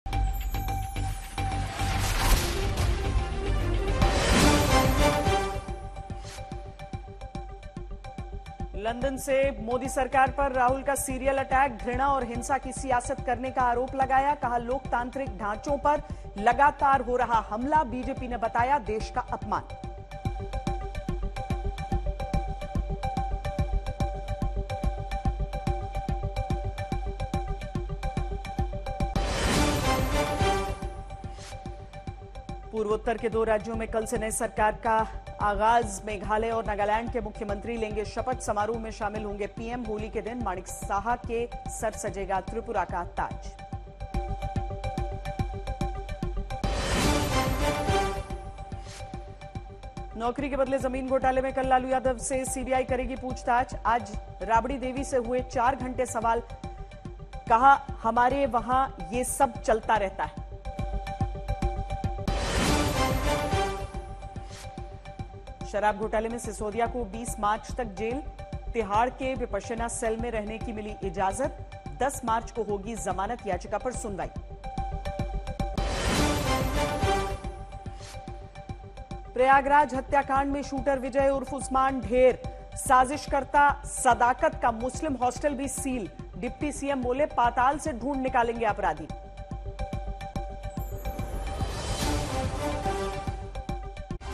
Top Headlines Of the day: Manish Sisodia | CBI | Rabri Devi House | BJP Vs AAP | Bihar Politics
शऱाब घोटाले में सिसोदिया को 20 मार्च तक जेल, तिहाड़ के बिपश्यना सेल में रहने की मिली इजाजत, 10 मार्च को होगी जमानत याचिका पर सुनवाई.

In the liquor scam, Sisodia got permission to stay in Tihar's Bipassana cell till March 20, hearing on bail plea will be held on March 10.